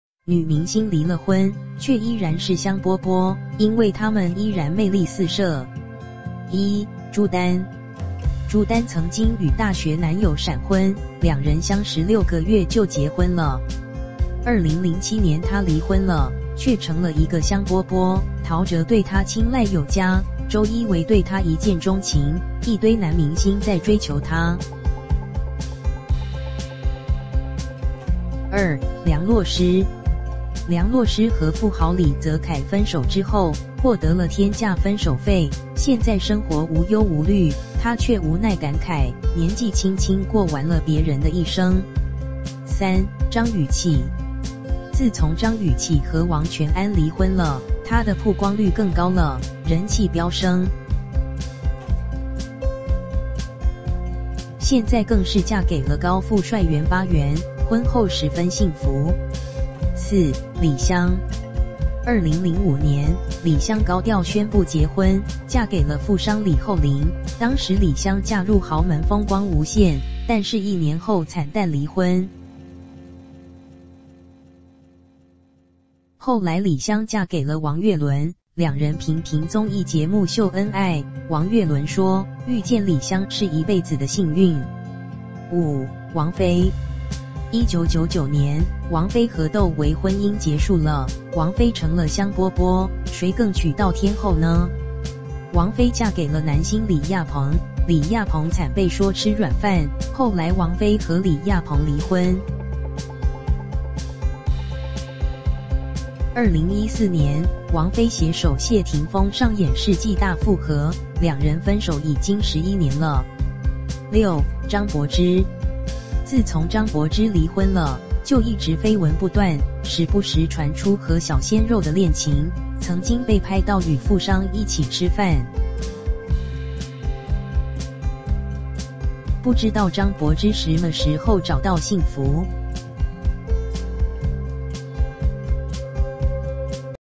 娱乐圈离婚了，却成了男人中“抢手货”的6大女星！
女明星离了婚，却依然是香饽饽，因为她们依然魅力四射。

1、朱丹

朱丹曾经与大学男友闪婚，两人相识6个月就结婚了。2007年她离婚了，却成了一个香饽饽，陶喆对她青睐有加，周一围对她一见钟情，一堆男明星在追求她。

2、梁洛施

梁洛施和富豪李泽楷分手之后，获得了天价分手费，现在生活无忧无虑，她却无奈感慨，年纪轻轻过完了别人的一生！

3、张雨绮

自从张雨绮和王全安离婚了，她的曝光率更高了，人气飙升。现在更是嫁给了高富帅袁巴元，婚后十分幸福。

4、李湘

2005年，李湘高调宣布结婚，嫁给了富商李厚霖，当时李湘嫁入豪门风光无限，但是1年后惨淡离婚。后来李湘嫁给了王岳伦，两人频频综艺节目秀恩爱，王岳伦说，遇见李湘是一辈子的幸运。

5、王菲

1999年，王菲和窦唯婚姻结束了，王菲成了香饽饽，谁更娶到天后呢？王菲嫁给了男星李亚鹏，李亚鹏惨被说“吃软饭”，后来王菲和李亚鹏离婚。2014年，王菲携手谢霆锋上演“世纪大复合”，两人分手已经11年了。

6、张柏芝

自从张柏芝离婚了，就一直绯闻不断，时不时传出和小鲜肉的恋情，曾经被拍到与富商一起吃饭。不知道张柏芝什么时候找到幸福~~